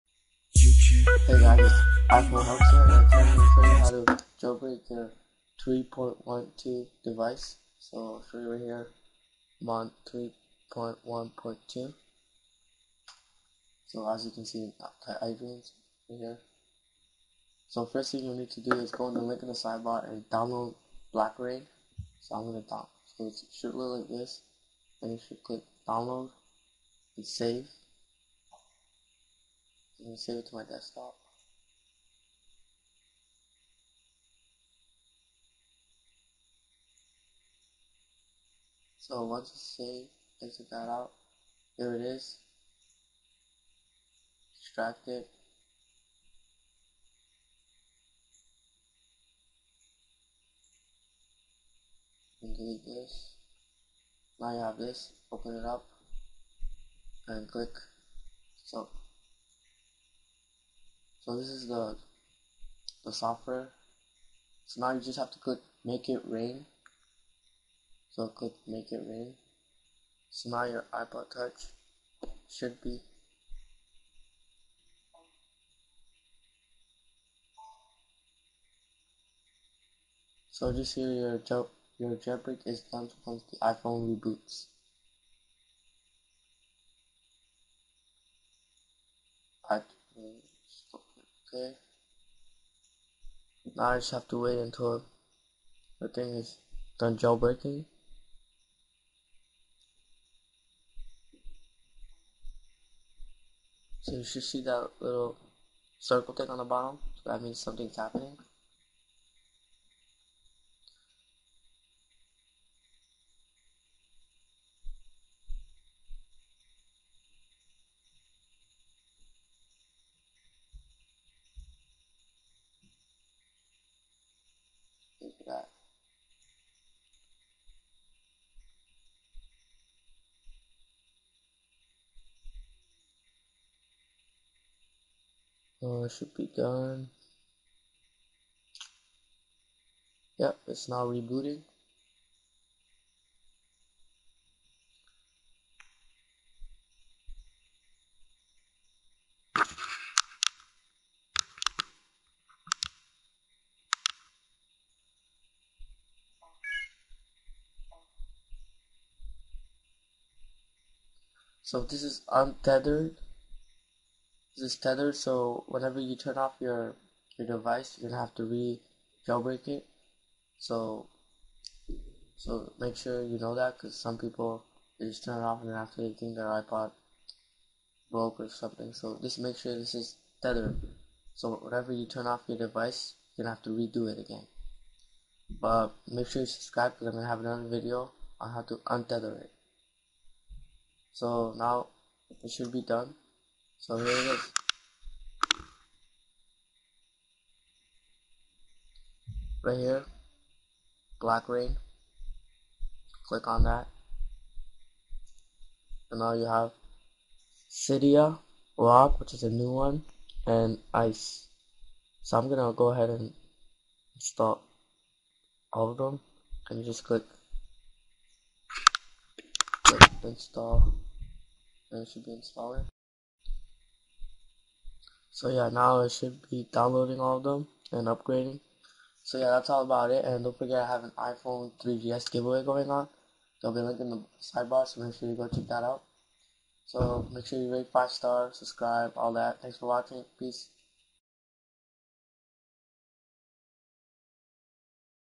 Easy Way How To Jailbreak NEW 3.1.2 Firmware For All Devices Windows Blackra1n!!
PLEASE READ

STEP 1 You must open up iTunes connect your device via usb and click restore & update to the NEW 3.1.2 Firmware

WARNING This will update your baseband so if you need to unlock do not do this on iPhone 3G & 3GS

Now Follow my instructions

Blackra1n works for: iPhone, 3G, 3GS, iPod Touch 1st 2nd & 3rd Gen

WARNING If you are jailbreaking iPod Touch 3rd Gen this is tethered so every time you reboot you will need to run blackra1n agian.